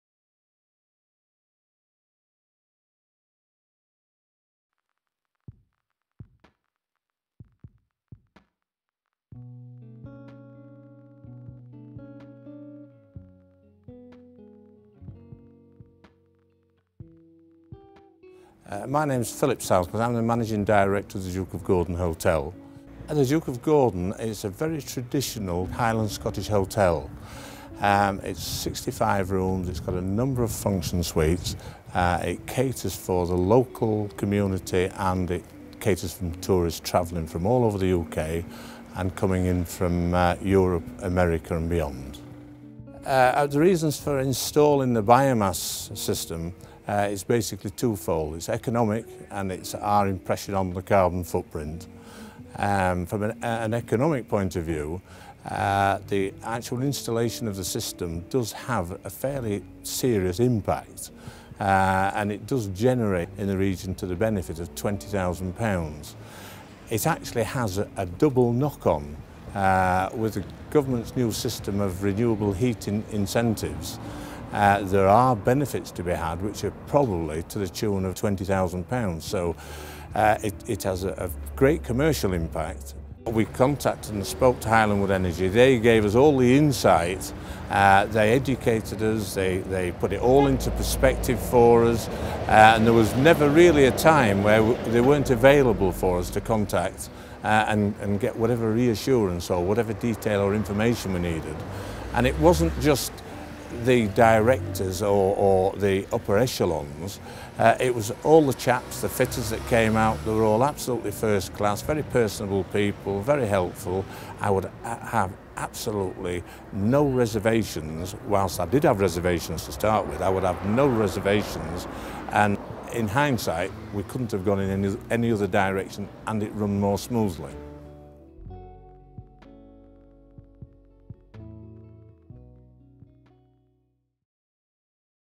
Duke of Gordon Hotel, Kingussie - Biomass heating system
Biomass wood fuel heating system at the Duke of Gordon Hotel in Kingussie